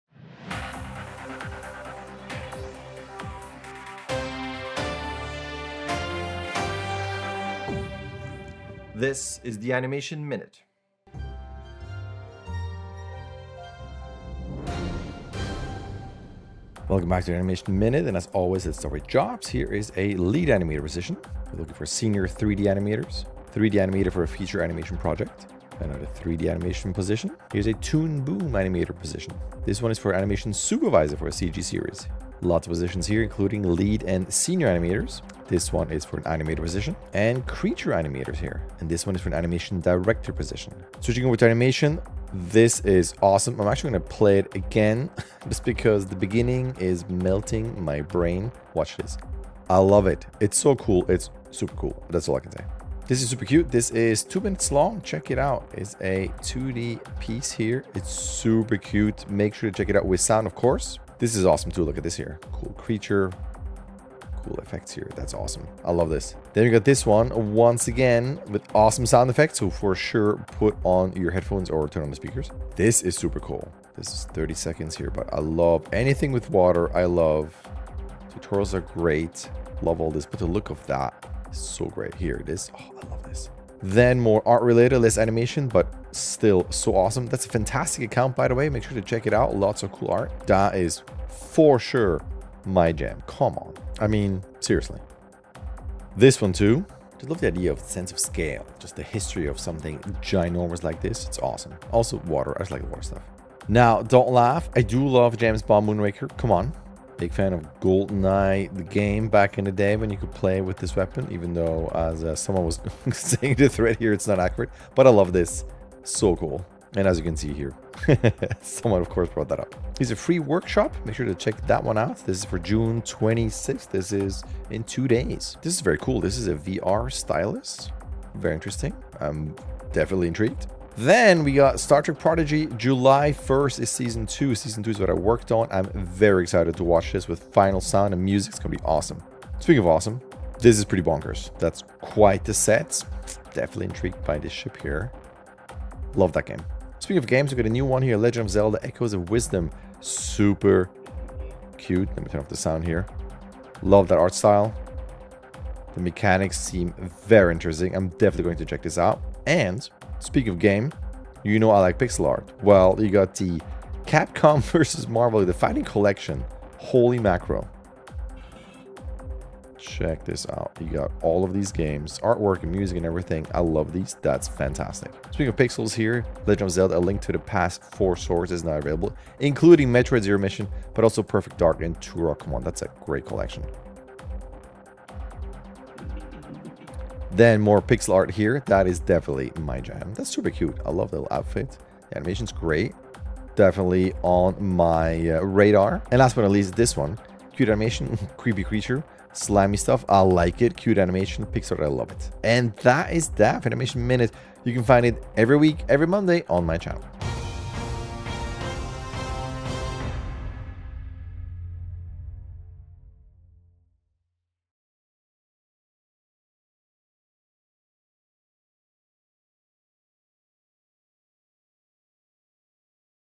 The Animation Minute: Weekly News! Jobs! Demo Reels and more! (June 17 - June 23, 2024)
Below is your weekly summary of all things animation!

Links to all the items I mentioned:
0:00 Intro
0:17 News

3:27 Outro
3:31 Would You Like To Know More?

✔️ Animation Director, Lead, Animator || Blizzard Cinematics, Paramount/Nickelodeon, Warner Brothers Animation and Industrial Light & Magic (19 years)